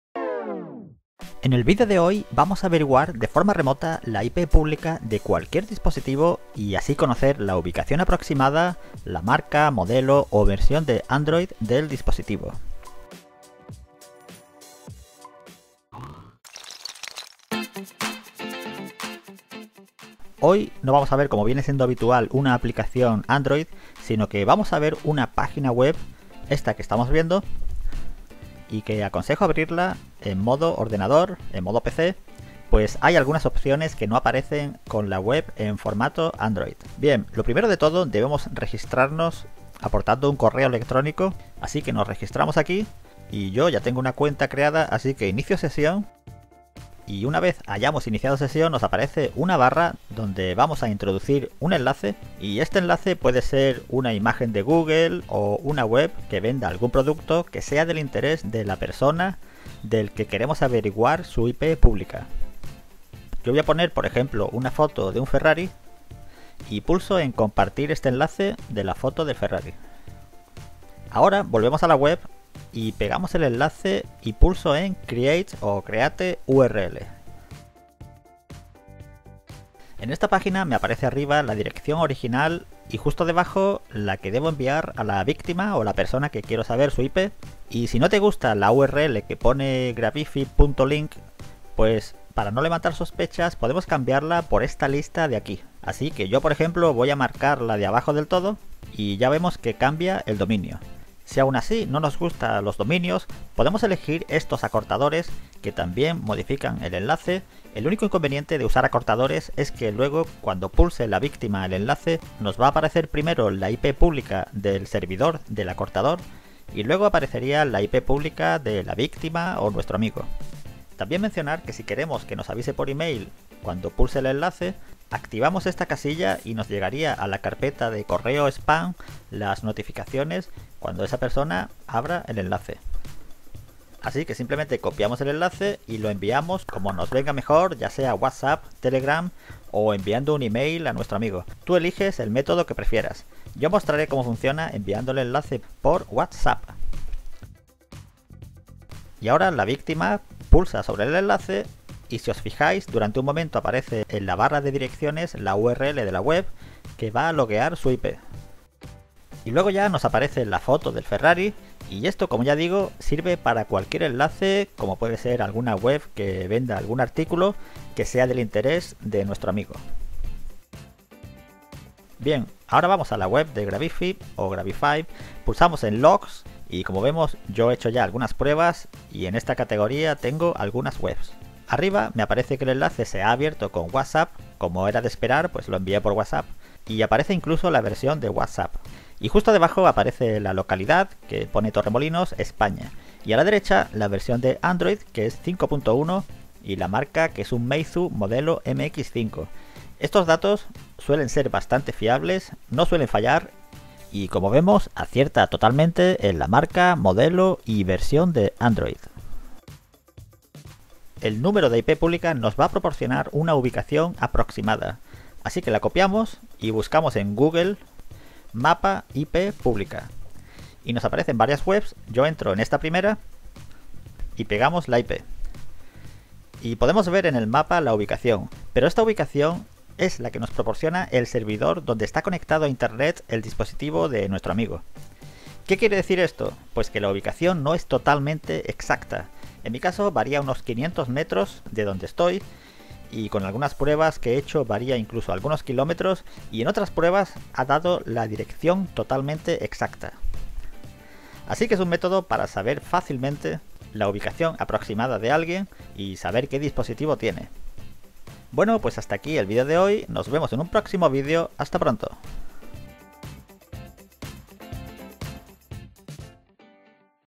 Saber la IP Pública de tu Teléfono | Ubicación y Modelo Dispositivo de forma remota
Conocer la IP pública nos permite saber algunos datos sobre un dispositivo como la ubicación aproximada y otros datos como el modelo de dispositivo y aplicaciones a las que se conecta a internet.